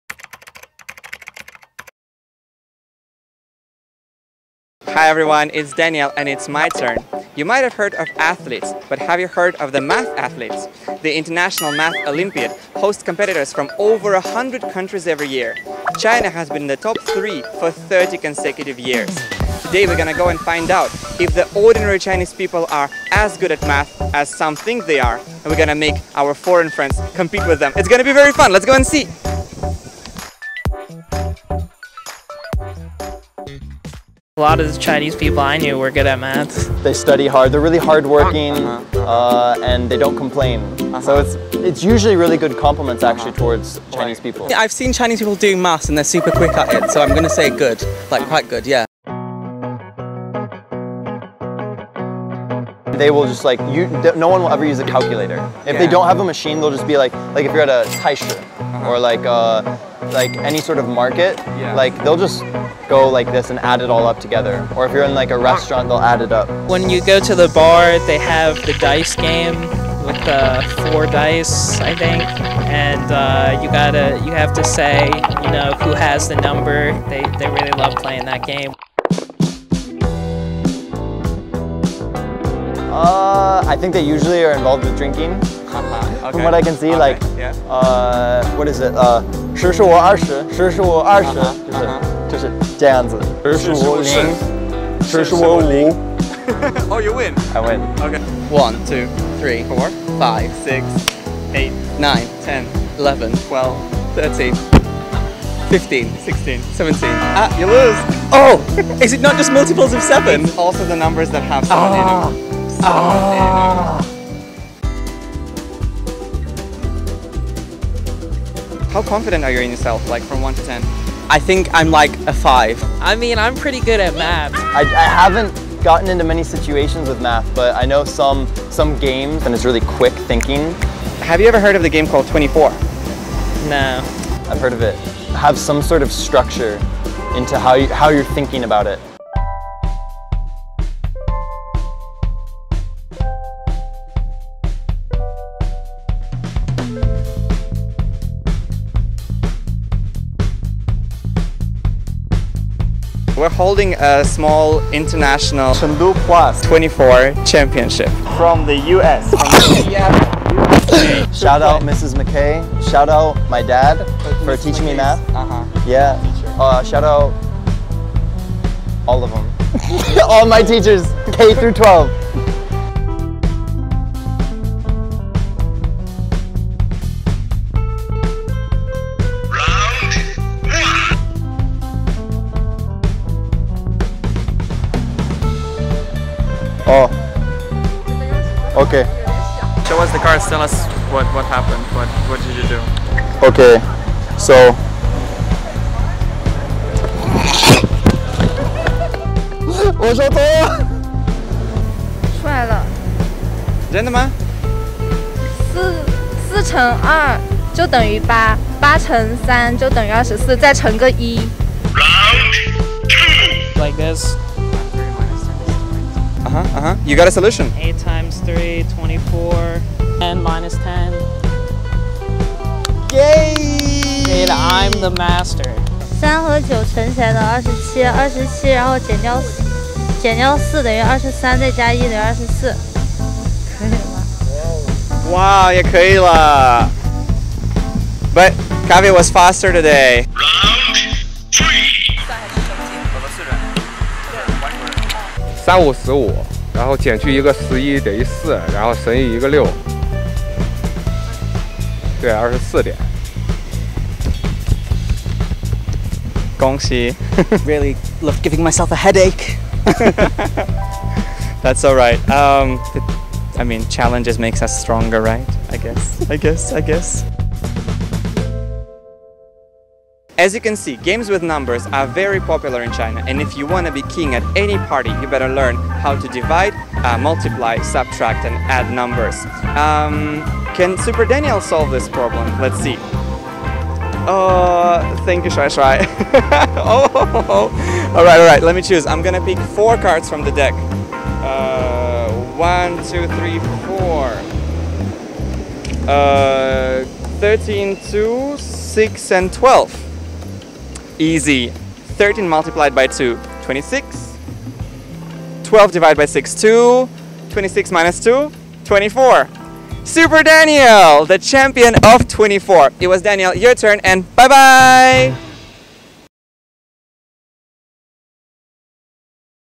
中国人数学好？外国人数学差？幻觉还是真相？|Chengdu Plus
每年举办的国际数学奥林匹克竞赛中，会有数百个国家参赛，中国已经连续三十年位居前三。让我们走到街头调查下是不是中国的普通人也是数学大神，让他们和外国朋友一起玩数学游戏大比拼，看看中外人民的数学能力差距！